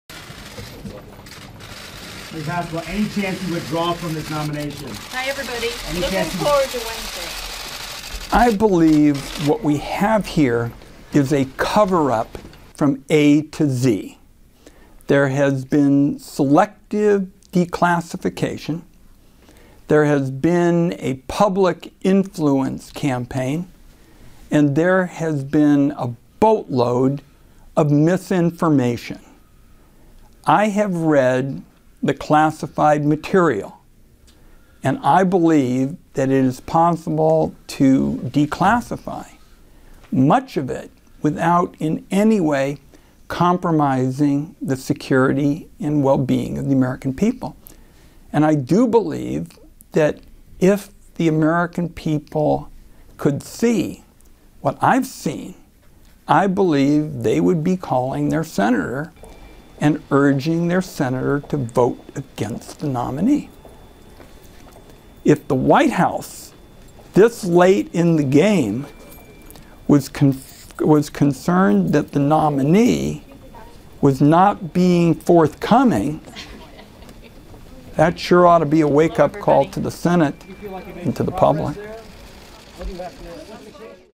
Trump's CIA Nominee in for Bruising Hearing
Gina Haspel is trying to convince Democrats that she's the right person to lead the CIA. But some, like Sen. Ron Wyden aren't convinced. He and others want the CIA to declassify her role in interrogation practices that some consider torture. (May 7) -- The Associated Press